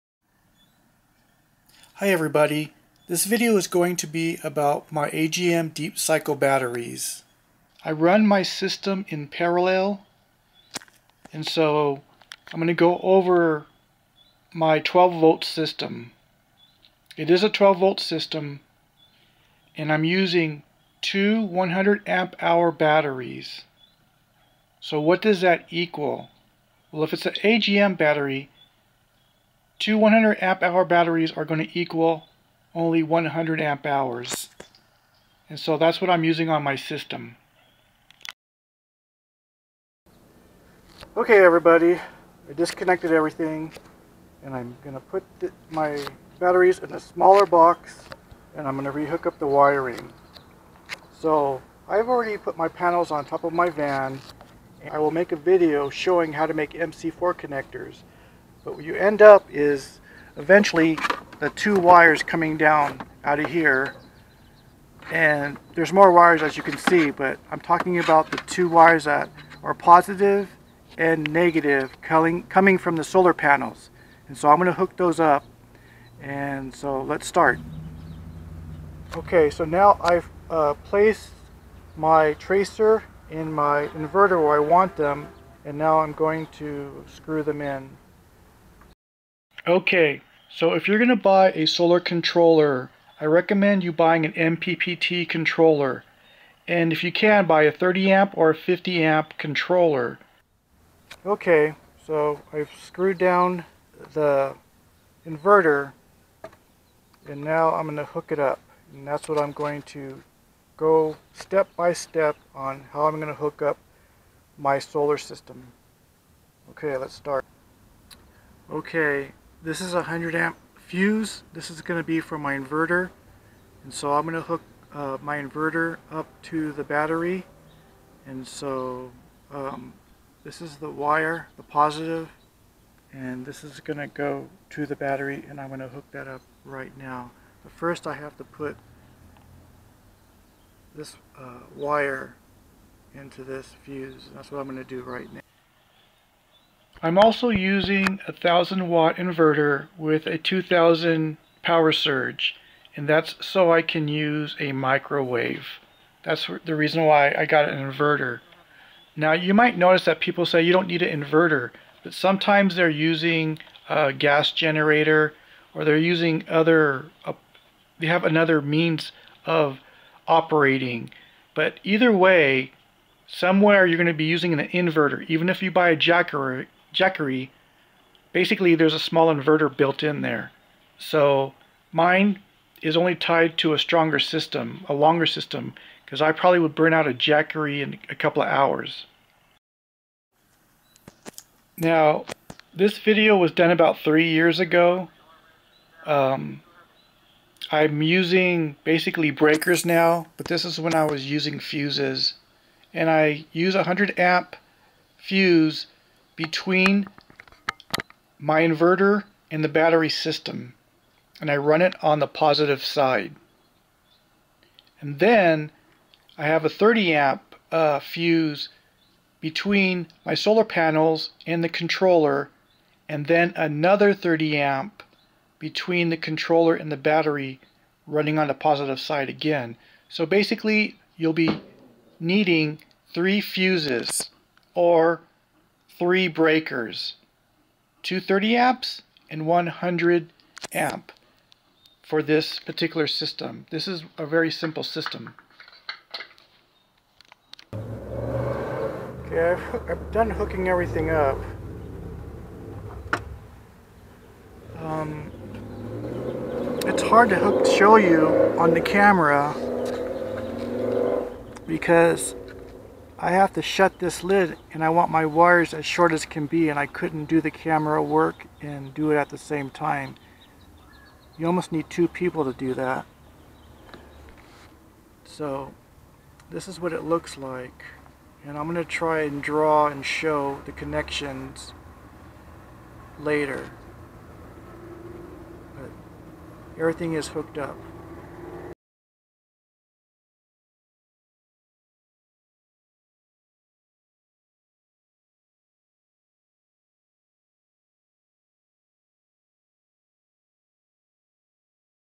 Solar Battery System | Leisure Battery | House Battery | AGM Deep Cycle Battery Still Working
Solar Battery System | Leisure Battery | House Battery | AGM Deep Cycle Batteries Still Working
Current solar battery set up. This video shows what fuses or breakers to use and set up. My solar battery bank is in parallel. I'm using a MPPT solar charge controller. and Secondly, How I use a solar energy as an alternate power source. Renewable energy using the sun and a battery storage system. This is a deep cycle battery 12v setup, this is part of my campervan build. My AGM batteries are still going strong. They are 4 years old.
Solar Charge Controller
Pure Sine Wave Inverter
30 Amp Breaker
100 Amp Breaker